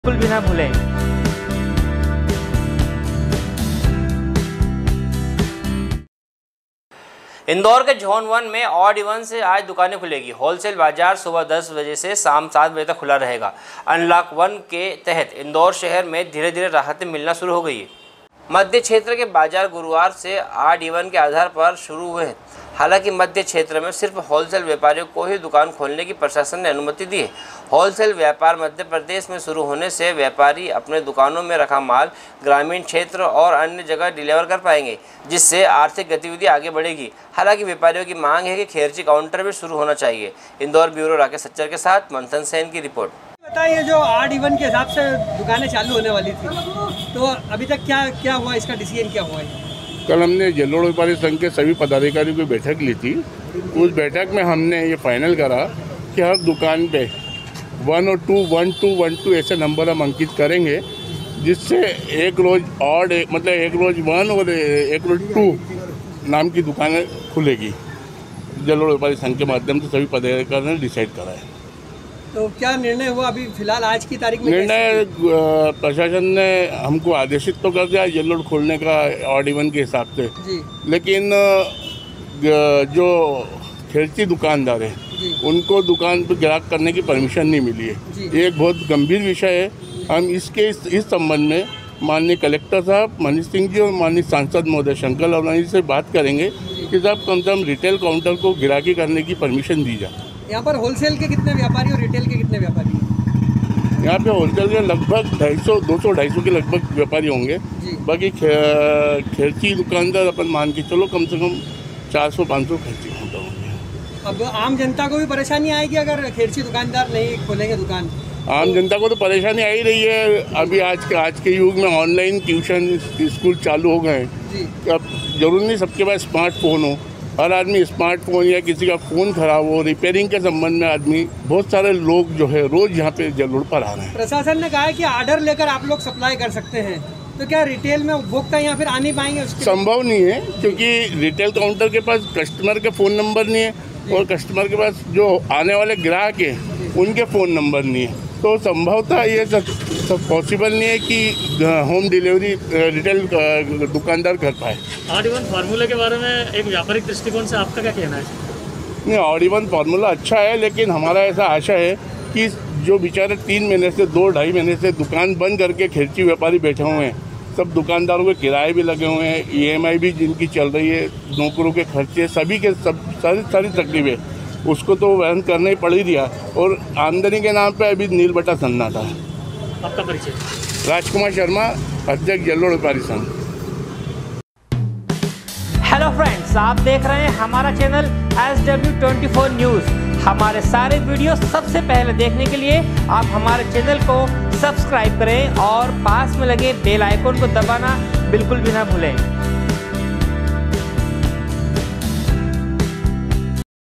SW 24 News Madhyapradesh#indore#11.06.2020
sw sw24news sw24newsmadhyapradesh livenews news24इन्दौर के झोन वन मे आड़ ईवन से आज दुकानें खुलेगी, होलसेल बाजार सुबह दस से शाम सात बजे तक खुलेंगां अनलॉक एक के तहत इंदौर शहर में धीरे धीरे राहत मिलती नजर आ रही है
इंदौर से ब्यूरो राकेश सच्चर के साथ मंथन सेन की रिर्पोट